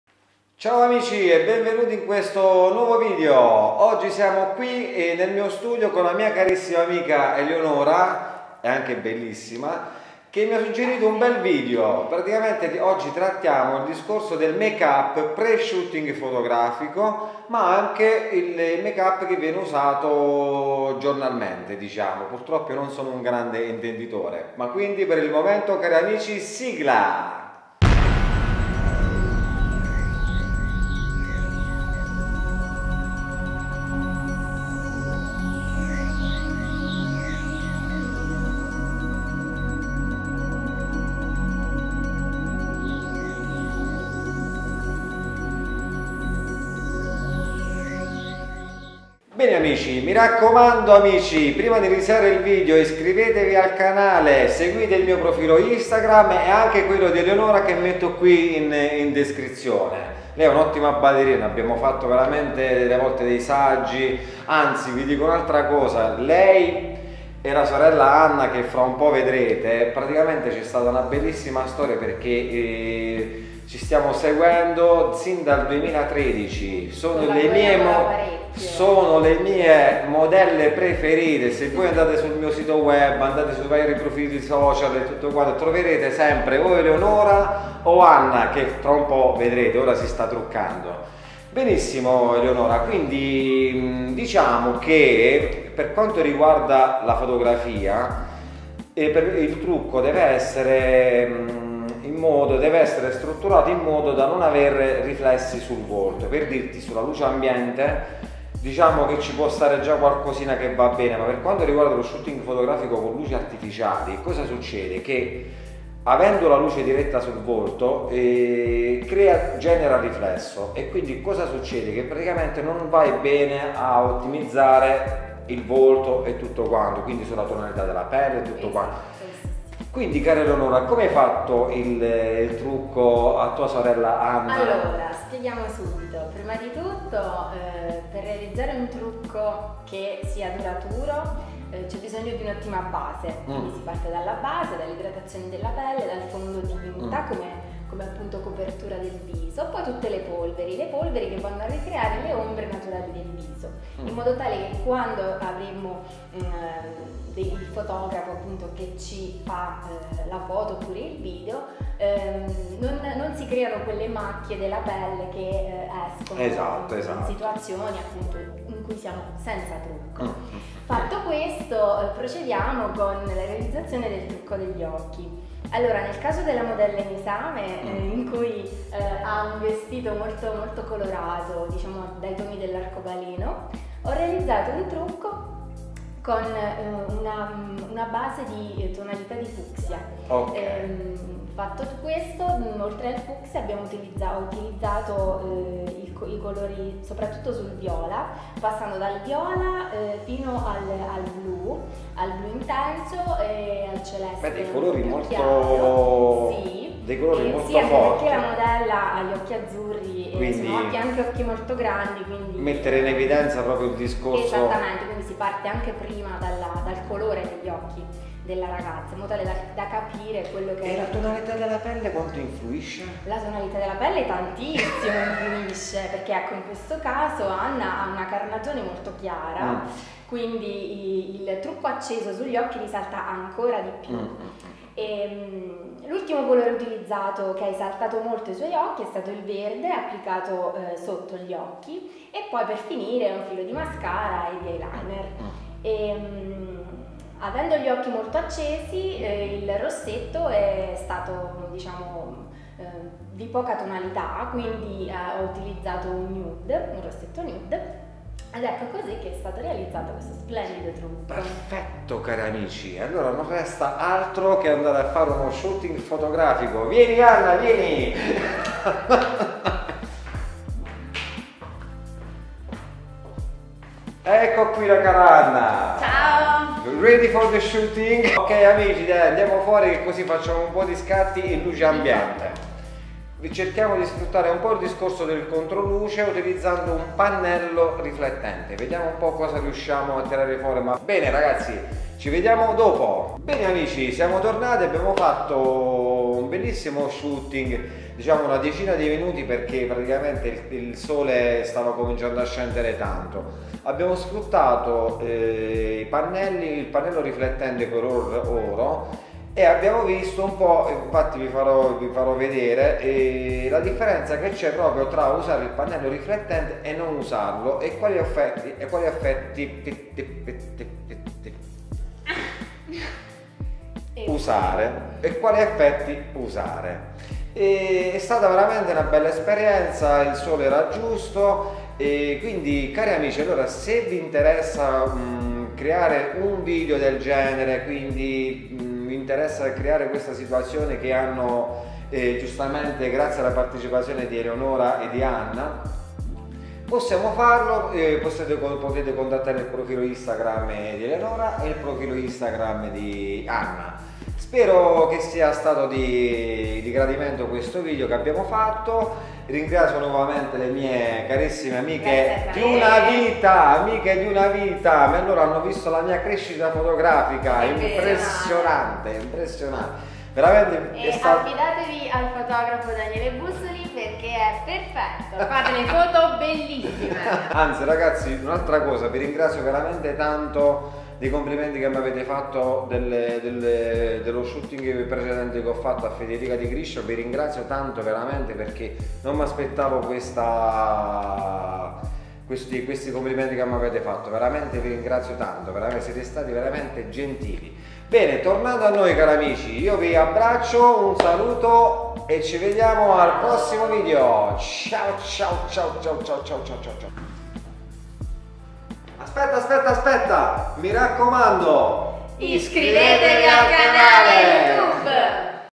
make up per fare le foto consigli pratici
Ciao amici in questo video le modelle anna e eleonora danno consigli su come truccarsi  prima di uno shooting fotografico e quali accorgimenti occorre per far si che lo shooting in collaborazione con il fotografo, si ottiene il risultato gradito